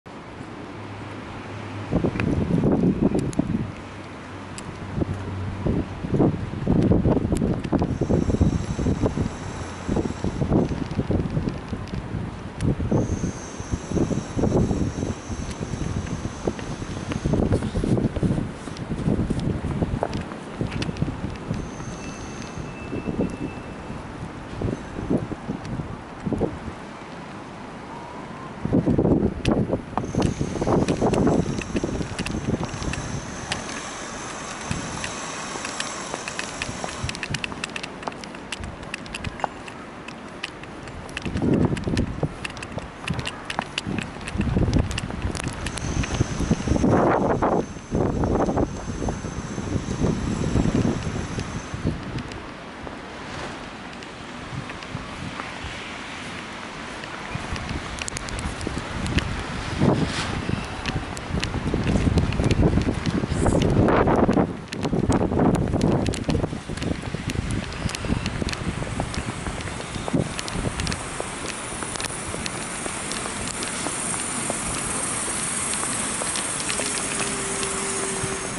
Winter
A winter day in my neighborhood.
The Beach, Toronto, Ontario, CANADA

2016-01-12